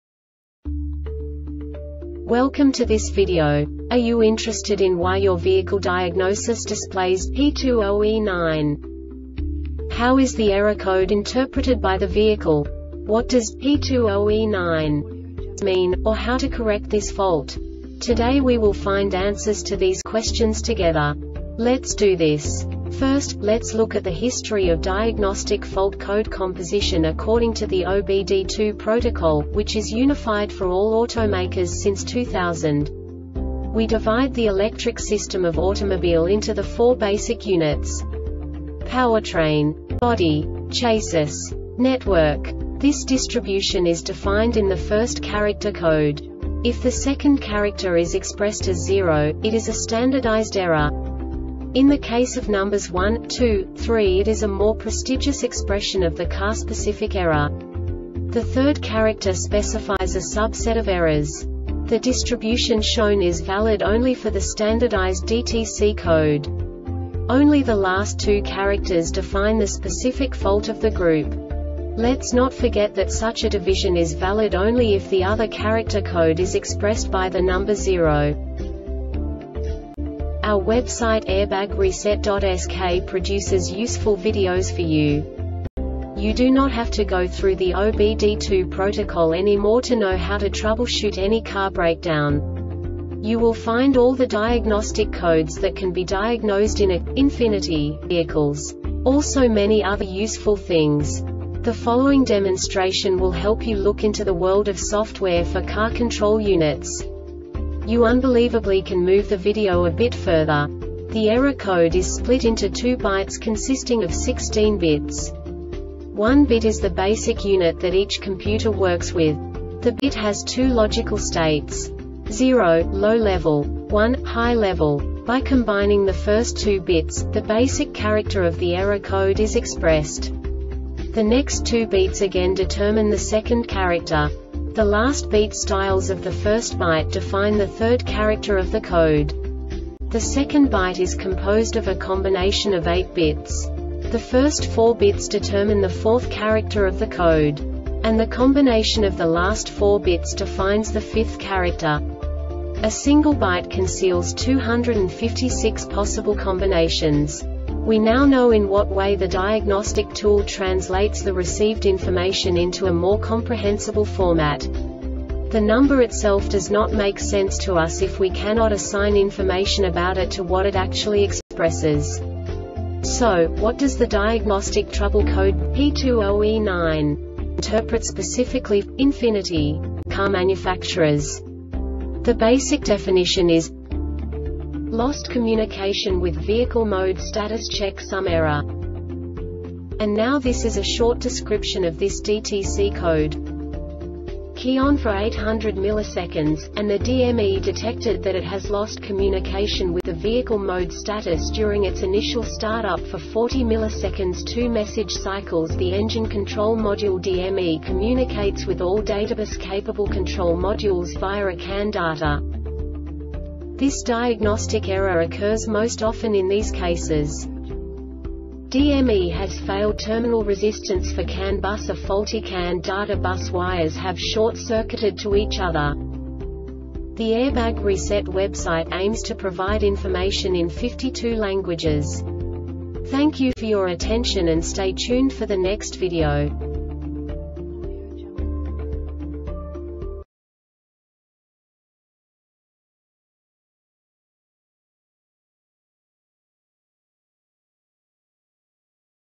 DTC Ford P20E9 Short Explanation
Contents:
0:21 Basic DTC analysis according to OBD2 protocol standard.
1:48 Insight into programming
2:58 A diagnostically understandable description of the Diagnostic Trouble Code
3:05 Basic error definition

Make:
Ford
Code:
P20E9
Definition: